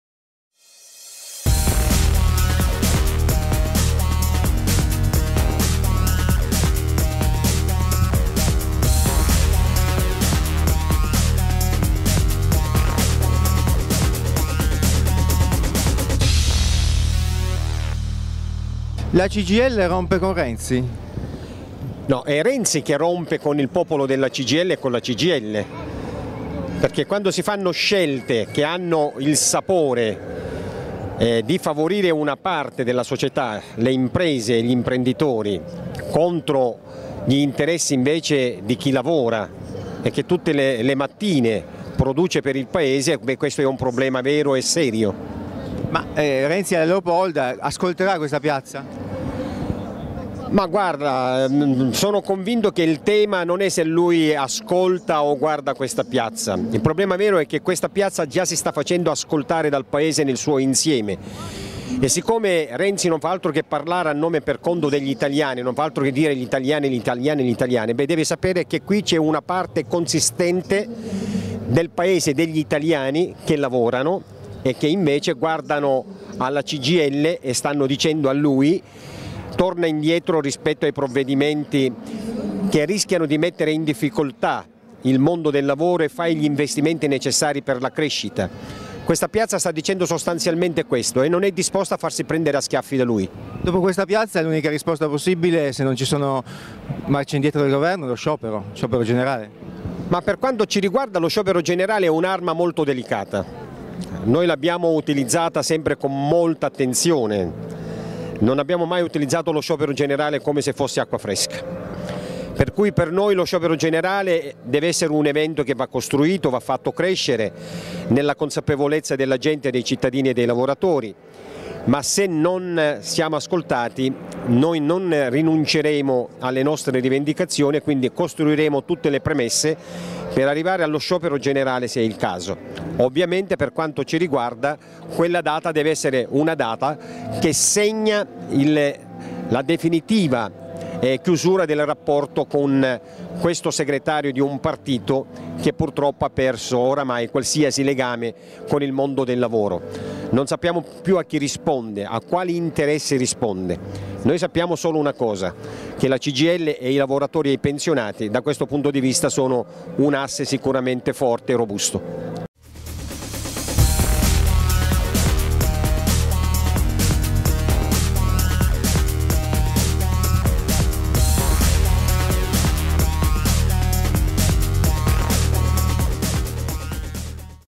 Cgil in piazza: NICOLOSI della CGIL
CGIL IN PIAZZA

Chi rappresenta questa piazza?

Intervista a Nicola Nicolosi della CGIL

25 ottobre 2015

servizio di Jacopo Venier

riprese di Roberto Pietrucci